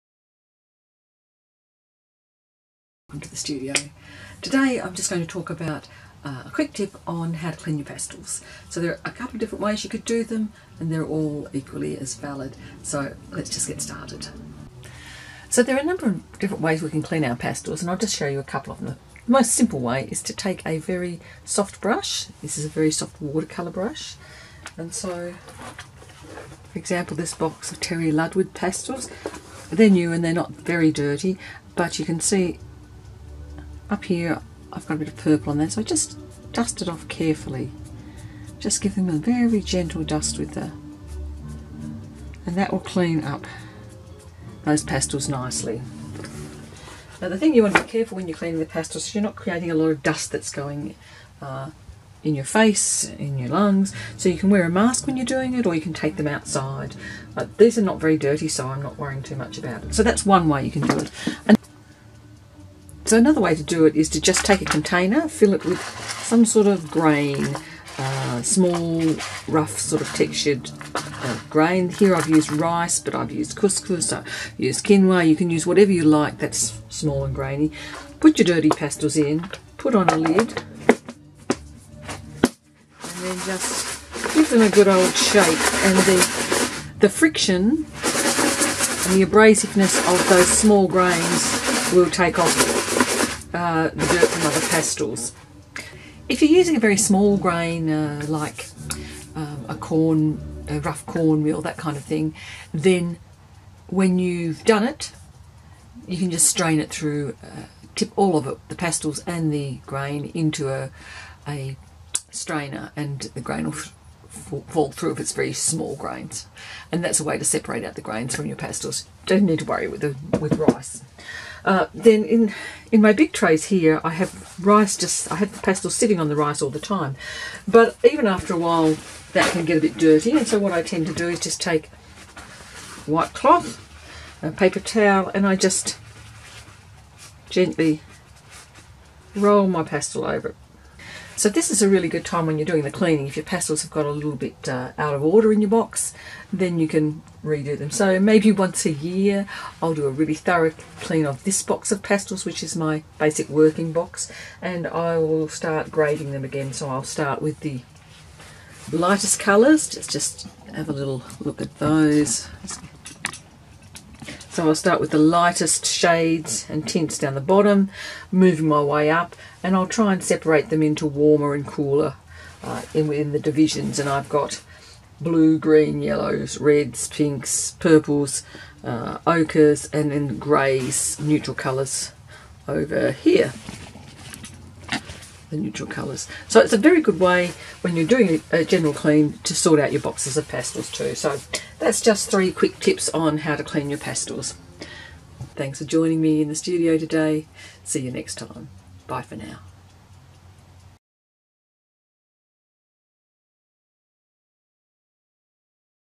How to clean soft pastels - Pastel painting course
Three simple methods to clean your pastels. Clean pastels will make your paintings sparkle so don't forget this important part of making art!

• My art is for sale on Etsy

Happy painting!